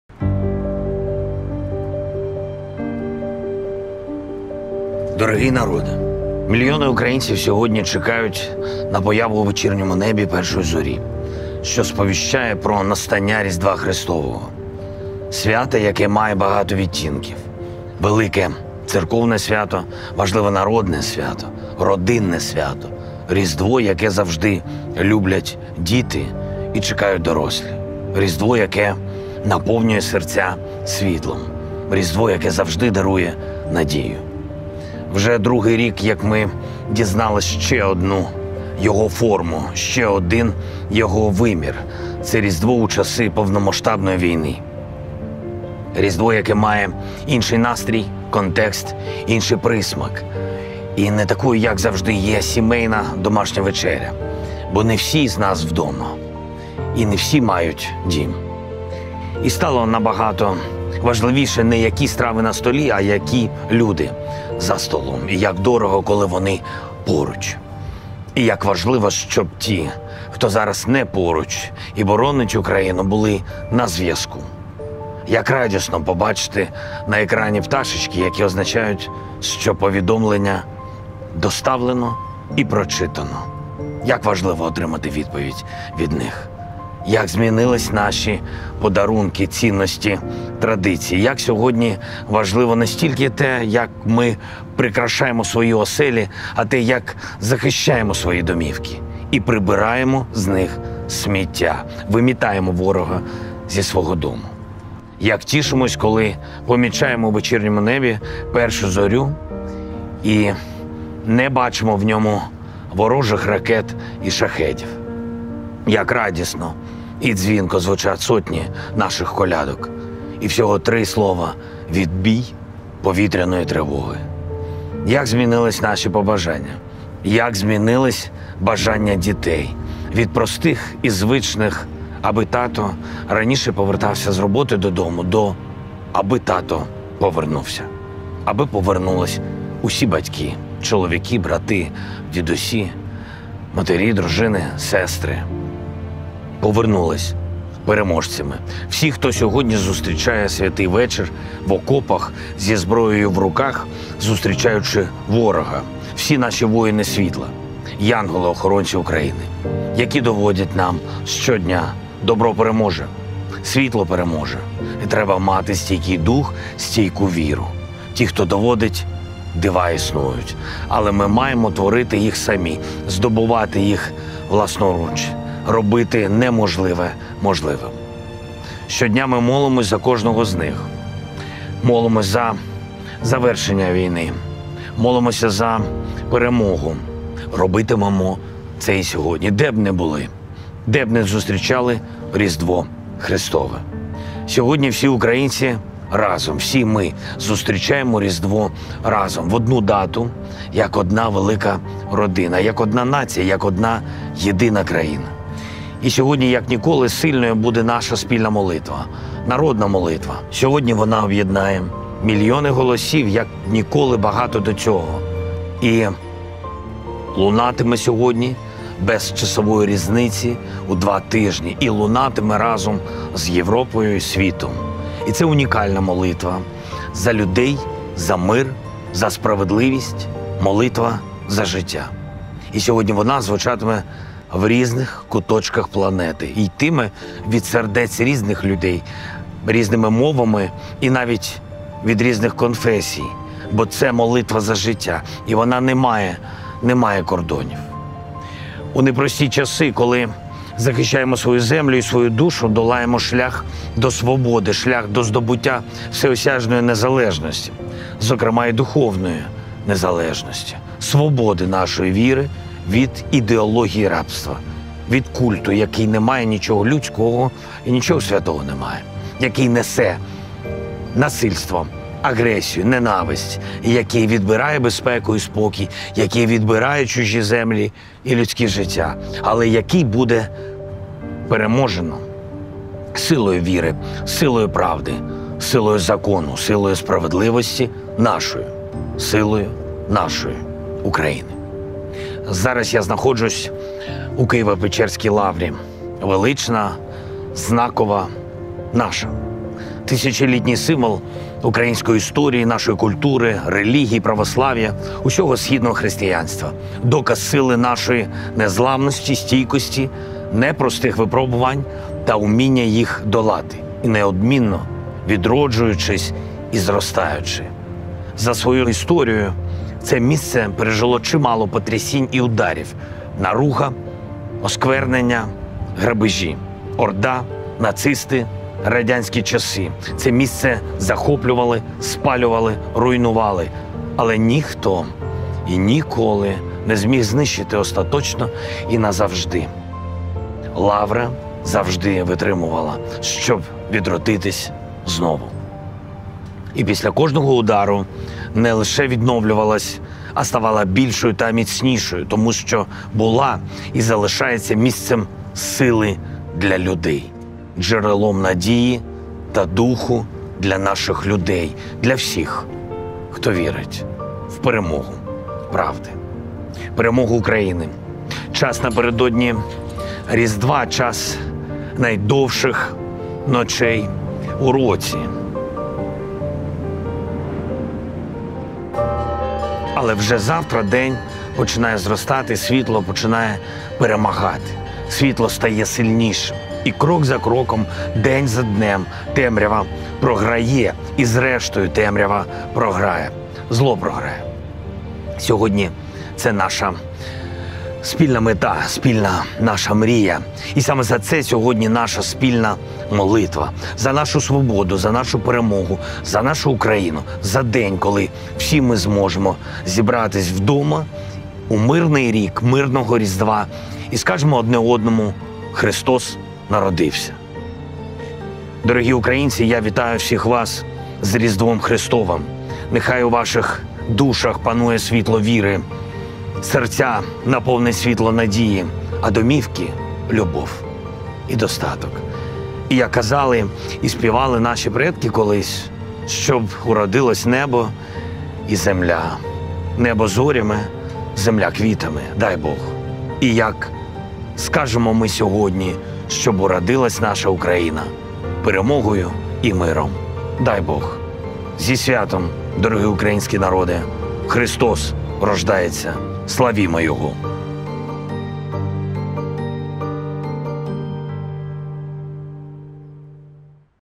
Поздравление Президента Украины с Рождеством Христовым
Время в канун Рождества – время самых длинных ночей в году. Но день уже начинает увеличиваться, свет начинает побеждать. Свет становится сильнее. И шаг за шагом, изо дня в день тьма проигрывает.

И в конце концов темнота проигрывает. Зло проигрывает.

Сегодня это наша общая цель, общая мечта. И именно за это сегодня наша общая молитва. За нашу свободу. За нашу победу. За нашу Украину.

Христос рождается! Славим его!

Поздравление Владимира Зеленского с Рождеством Христовым.